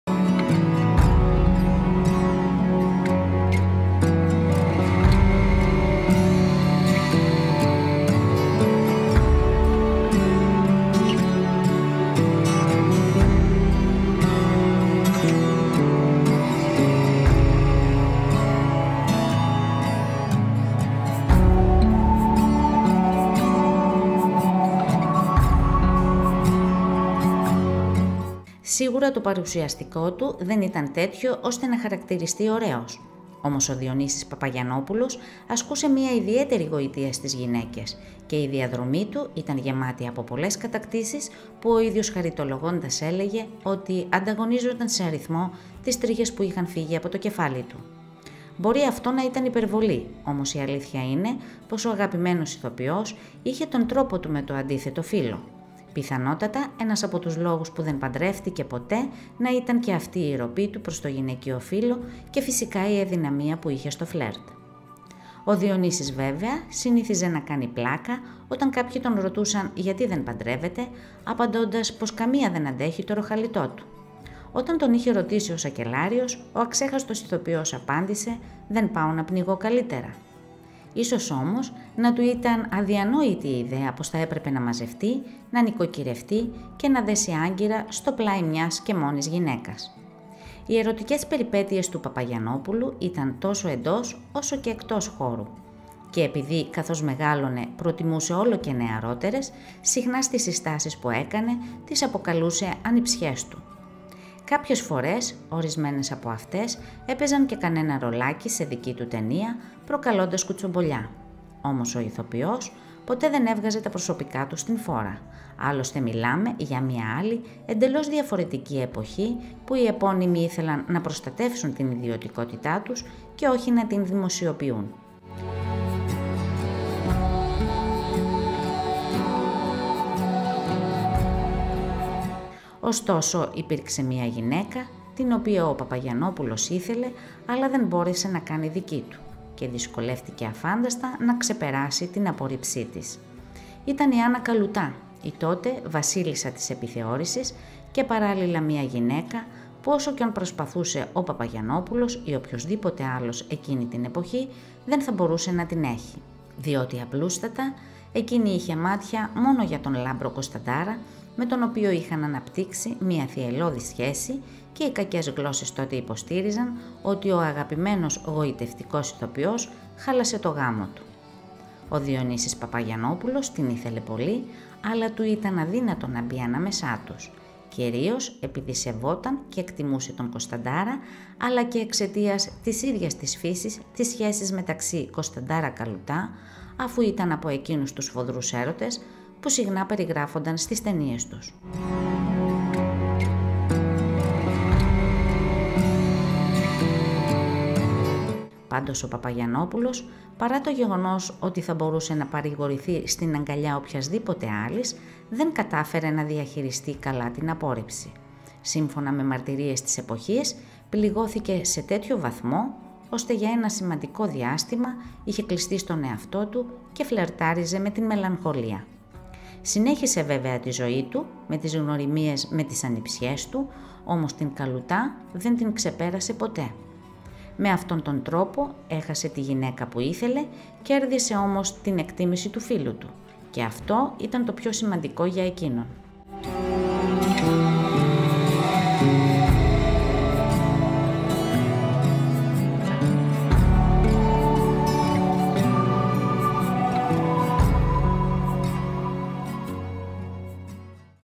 Διονύσης Παπαγιαννόπουλος: Ο τρελός έρωτας για μία συνάδελφό του, που τον έφτασε στη μελαγχολία!!
ΓΙΑ ΝΑ ΜΠΟΡΕΣΕΤΕ ΝΑ ΜΑΣ ΔΕΙΤΕ ΣΤΟ FACEBOOK ΚΑΝΤΕ ΚΛΙΚ ΕΔΩ